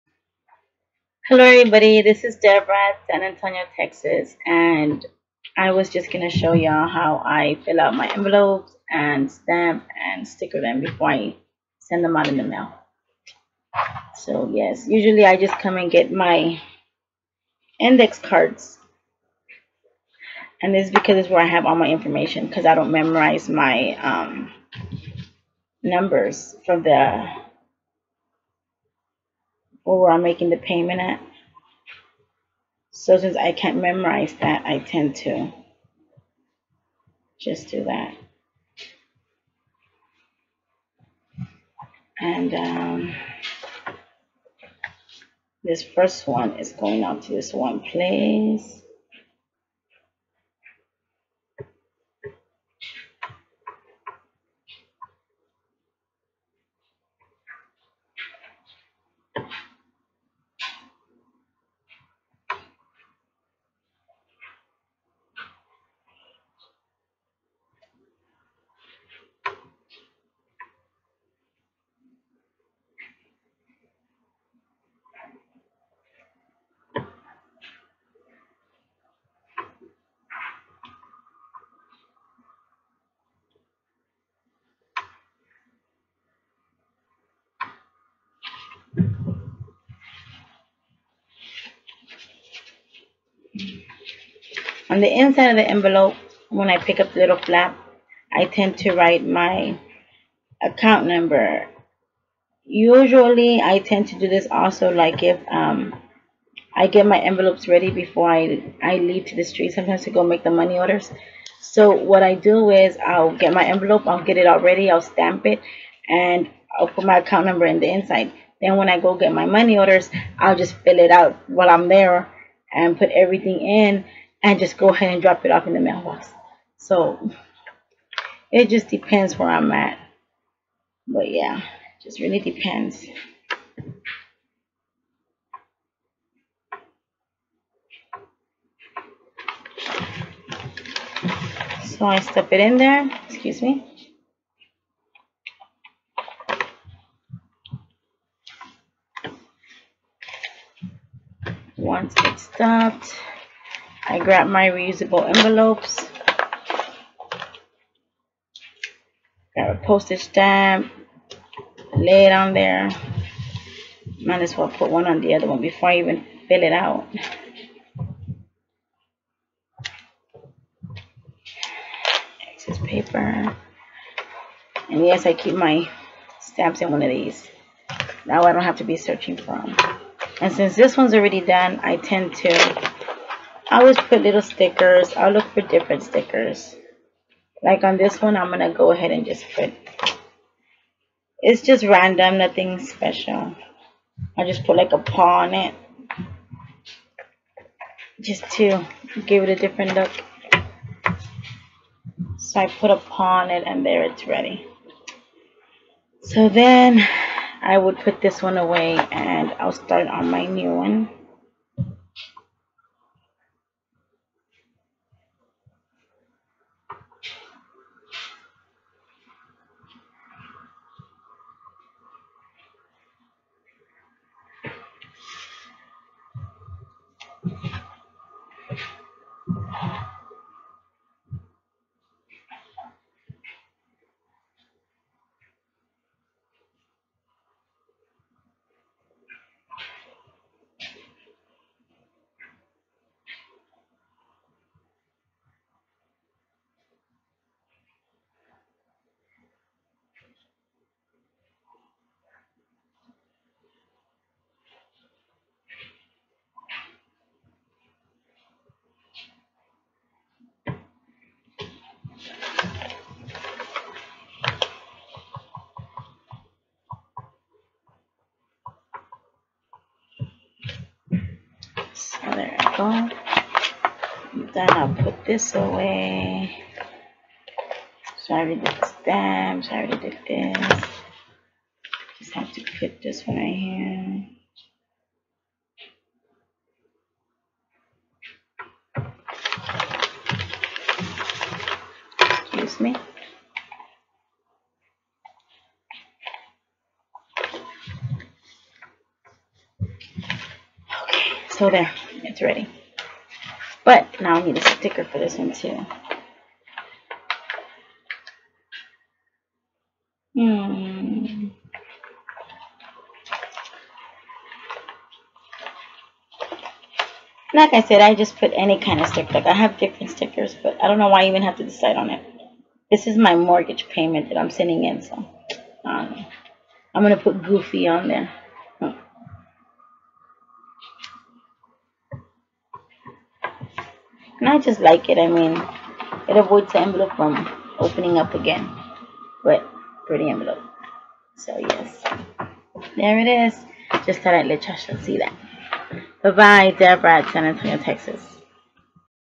Sending my payments via mail by Debra @S.A,TX
Just my routine of sending out mail.